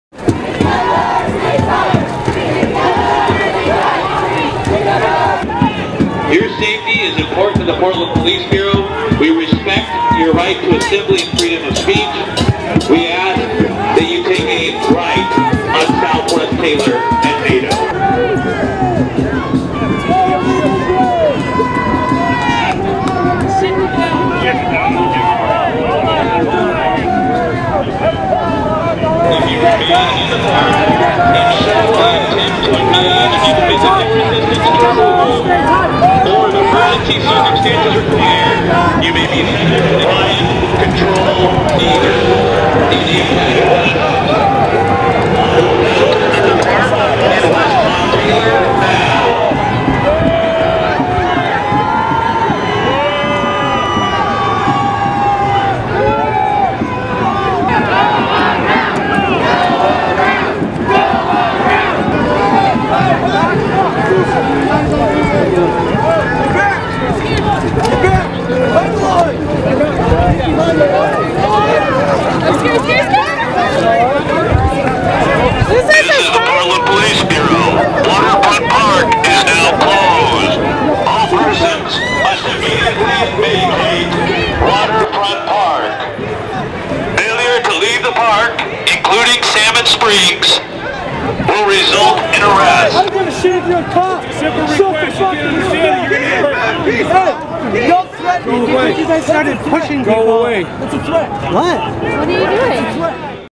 Waterfront Park - Portland - 2017
Anti-Trump protesters are shut out of Waterfront Park near the Morrison Bridge in Portland, Oregon on Inauguration Day (2017). The small group that made it through a gap in police lines and into the park before entry was cut off were almost kettled when more cops showed up near the on-ramp, leaving only one exit.